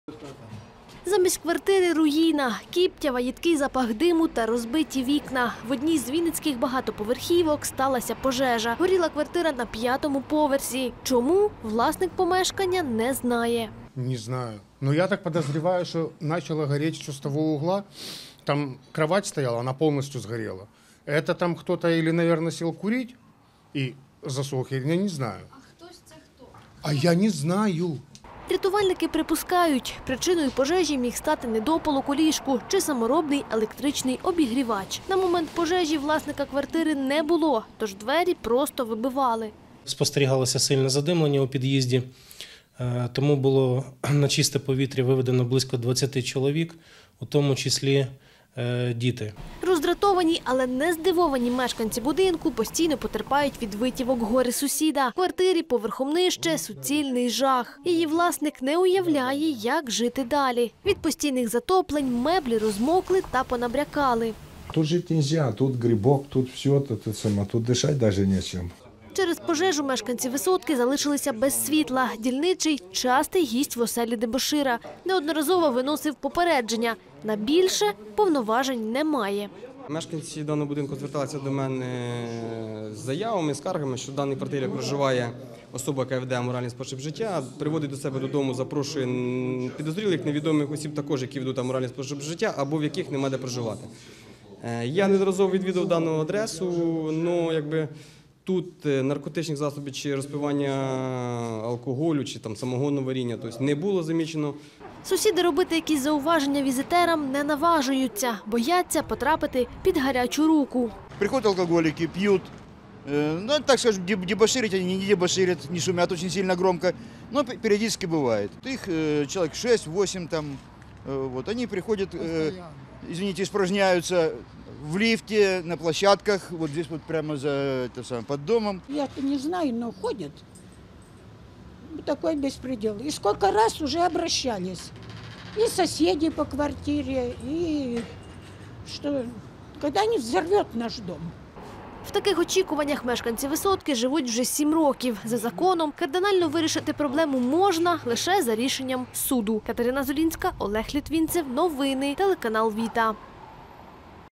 Горе-сусід тримає в страху всю багатоповерхівку, новини 2018-12-07
Коли від горе-сусіда страждає вся багатоповерхівка. На вулиці Чорновола в одній із квартир сталася пожежа. Власника на той момент вдома не було. А серед версій рятувальників щодо загоряння: або недопалок, або саморобний обігрівач. Обуренню мешканців немає меж. Кажуть чолов’яга своїми витівками тримає у страху весь будинок не один рік. То водою заливає, то друзів-дебоширів приводить.